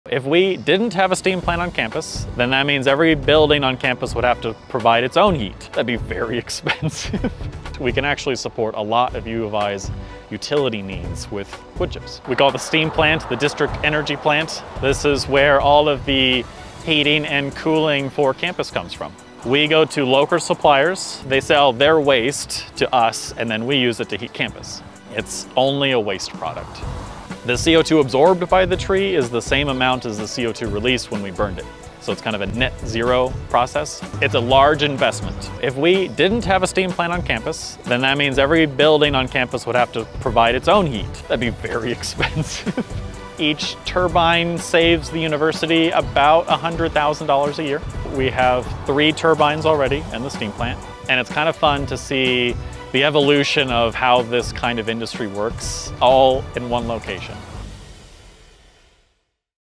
Campus energy on 🔥.
The University of Idaho’s District Energy Plant, located at the heart of campus, powers our buildings with innovative, eco-friendly technology. By using sustainably sourced wood chips, the Steam Plant reduces emissions and minimizes waste from local lumber mills, supporting both the environment and our community. This central energy system keeps costs down while heating and cooling campus efficiently, highlighting U of I’s commitment to sustainable energy and environmental responsibility.